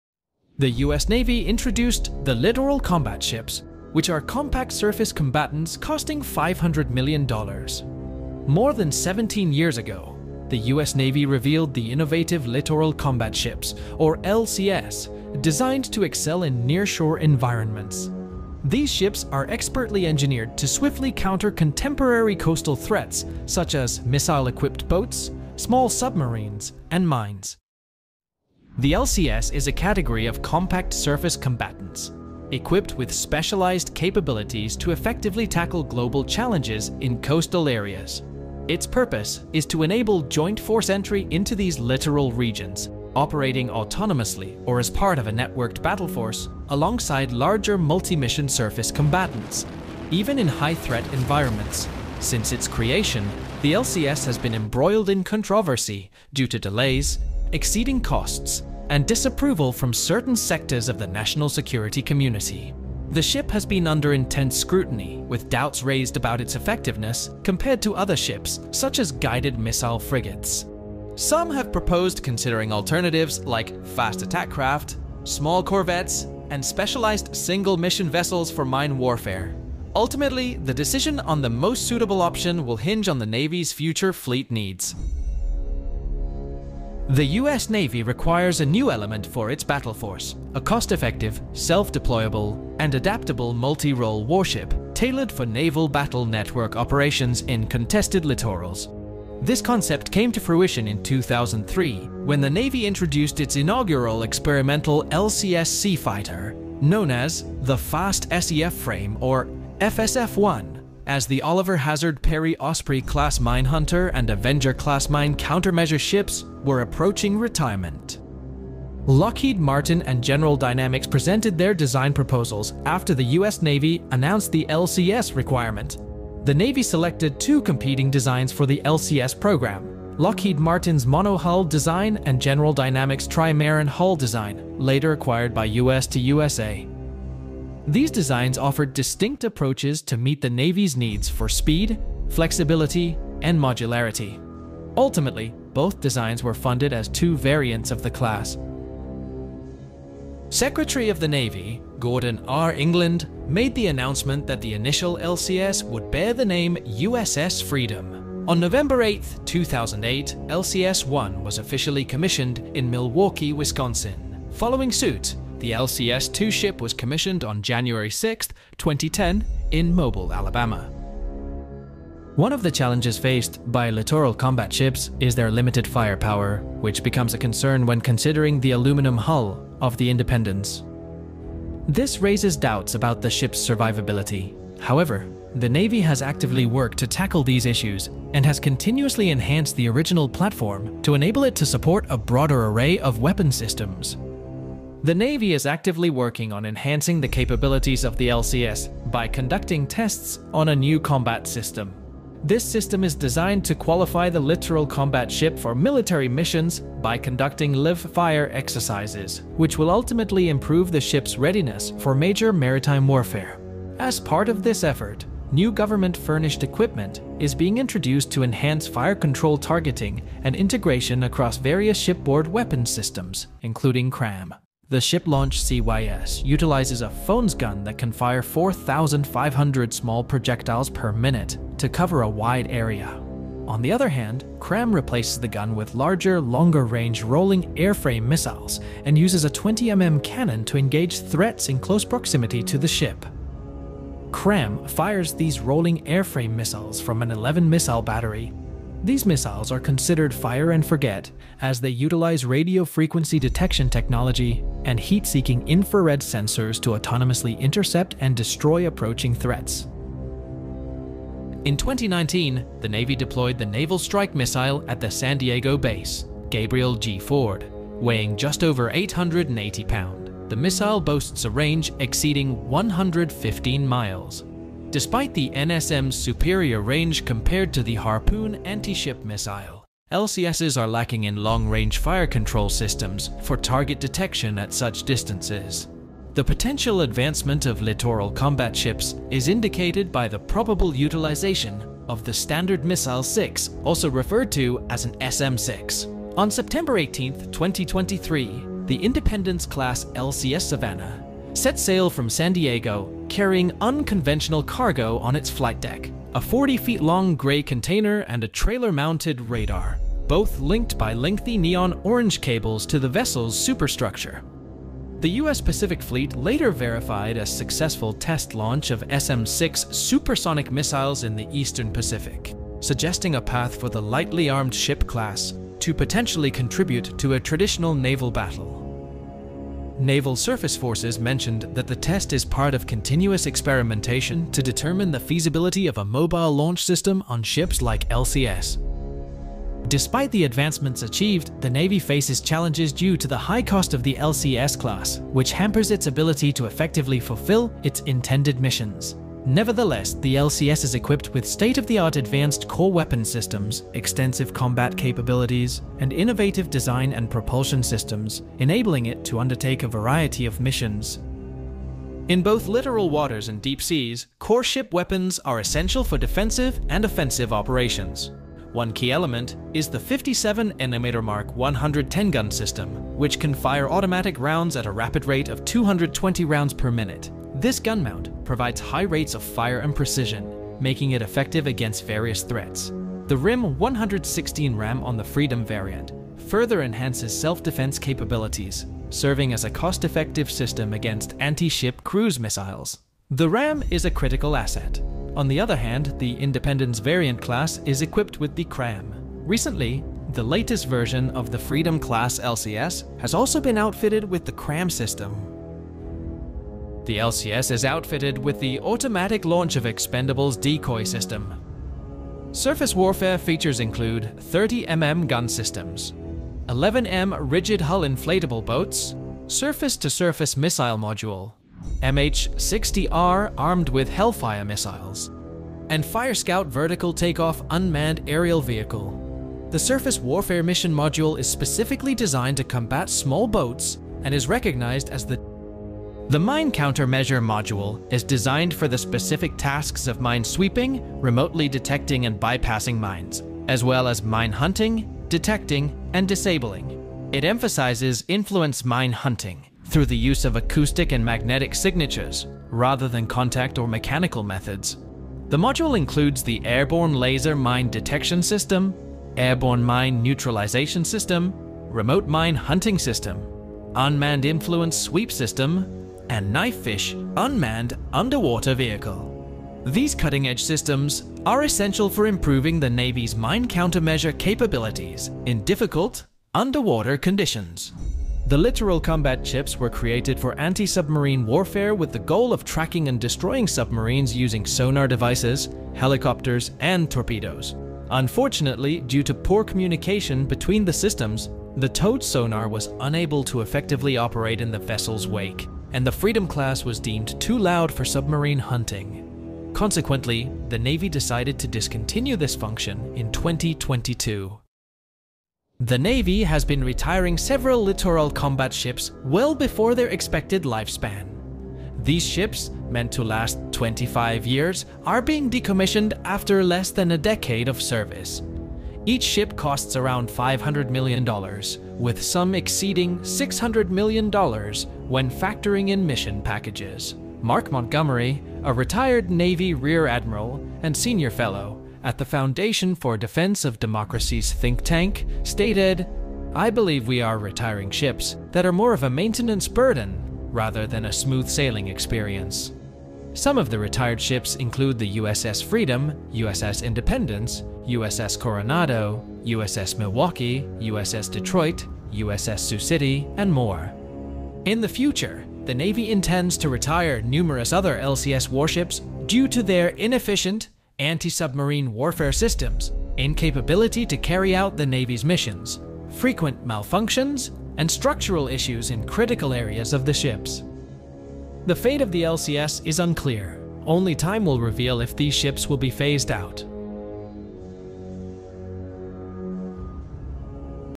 Discover the US Navy's $500 million warship, The Littoral Combat Ship
The US Navy introduced the Littoral Combat Ships (LCS) over 17 years ago, aiming to excel in Nearshore environments against contemporary Coastal threats such as missile-equipped boats, small submarines, and mines. These compact surface combatants, costing $500 million each, were designed to operate autonomously or as part of a networked battle force alongside larger multi-mission surface combatants, even in high-threat environments. The LCS program initially faced controversy due to delays, exceeding costs, and skepticism from certain sectors of the National Security Community.

Lockheed Martin and General Dynamics presented competing designs for the LCS program, resulting in two variants: Lockheed Martin's monohull design and General Dynamics' trimaran hull design. Both designs were funded as two variants of the class, with the USS Freedom and USS Independence being the first vessels commissioned in 2008 and 2010, respectively.

Despite advancements and continuous enhancements to address challenges like limited firepower and survivability concerns, the LCS has faced criticism for its high cost and early retirements due to maintenance burdens, frequent malfunctions, and structural issues. The Navy is actively working on enhancing the LCS's capabilities through new combat systems, including live-fire exercises to improve readiness for major Maritime Warfare.

However, the fate of the LCS remains uncertain as the Navy contemplates retiring numerous warships due to inefficient anti-submarine Warfare systems and incapability to carry out missions effectively. Only time will tell whether these ships will be phased out or further developed to meet evolving naval needs.